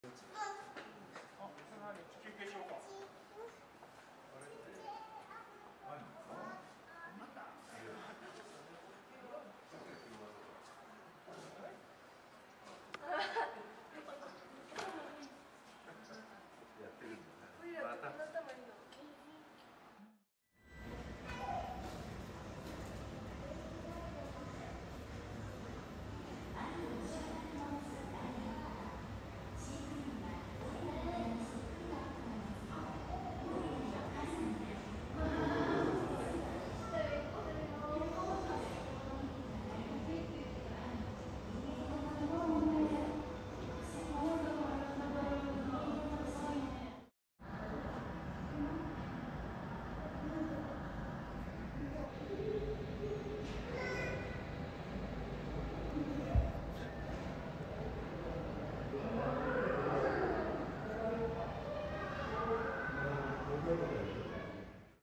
Mom Gorilla Plays With Her Daughter Who She Once Abandoned | The Shabani Family
Annie is a now 9-year-old girl born to Shabani (male) and Ai (female).
Because her mother Ai abandoned her, Annie was artificially nursed for about a year after her birth.
Since Annie returned to the flock, Ai and Annie have been getting along well.
It is said that Ai and Annie do not realize they are mom and daughter, but only they know that.
It is a fact that Ai and Annie are good friends.

This video was filmed at the Higashiyama Zoo 2021.